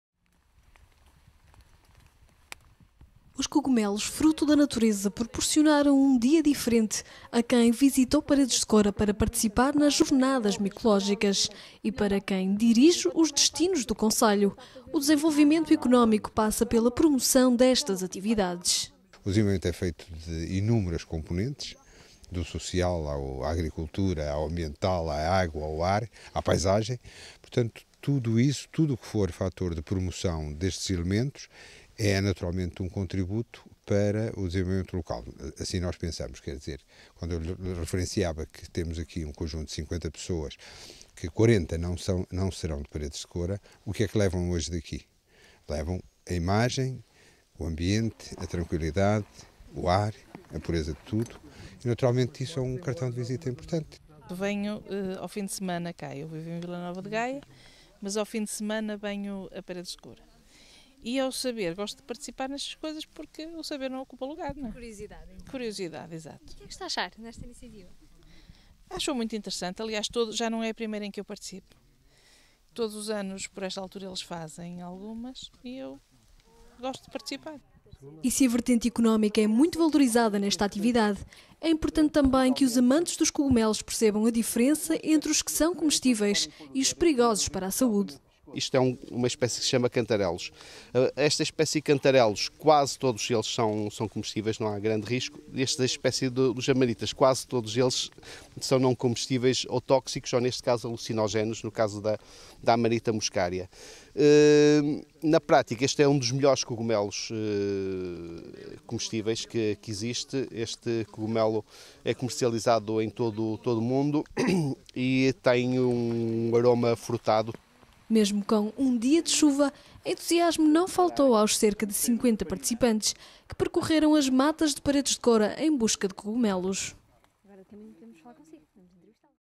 Jornadas Micologicas Paredes de Coura Porto Canal
Paredes de Coura dinamizou mais uma jornadas Micológicas, onde o cogumelo é rei e senhor.

Jornalista; Andreia Alexandra Fernandes
Imagem: José Ribeiro